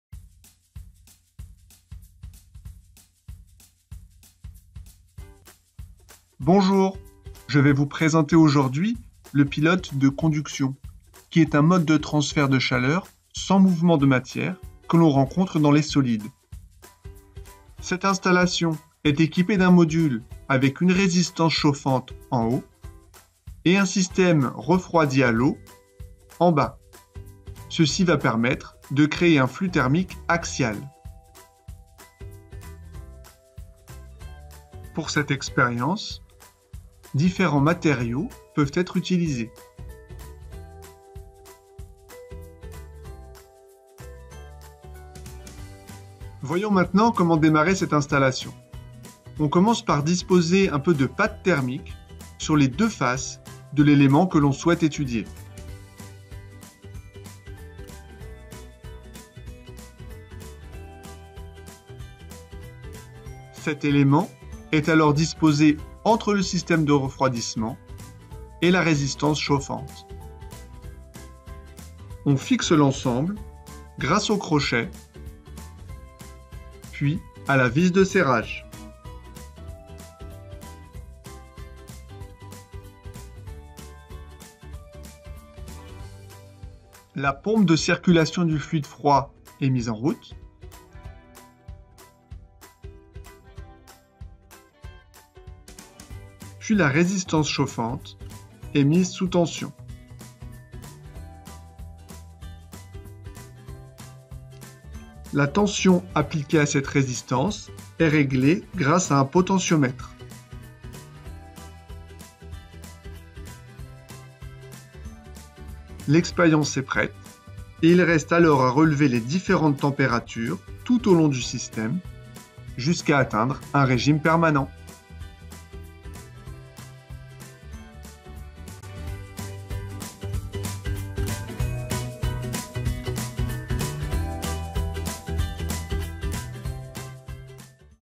Etude de la conduction thermique
Voici un tutoriel de démarrage du poste d'étude de la convection. Installation du département chimie de l'IUT d’Orléans.
 Bon visionnage